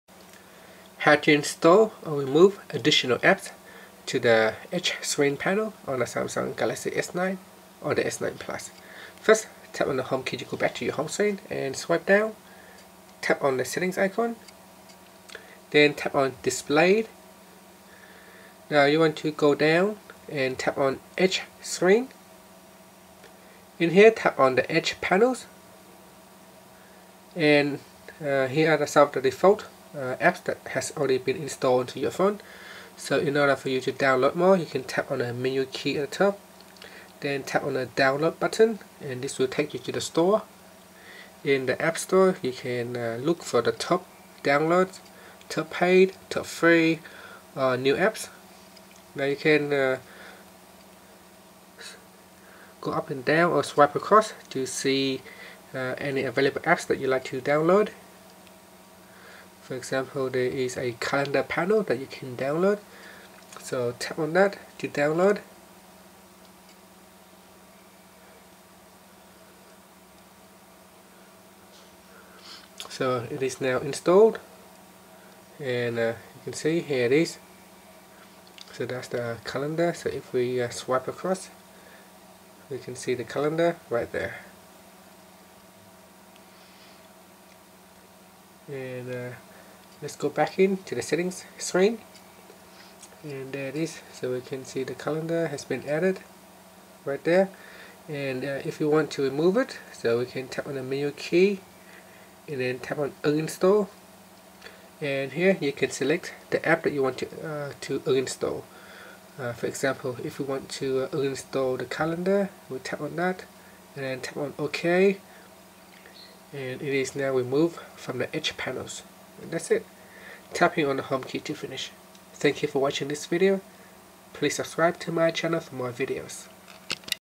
Samsung Galaxy S9 / S9+: Install / Remove Additional App to Edge Screen Panel
Learn how you can install or remove additional app tot he edge screen panel on Samsung Galaxy S9 / S9+.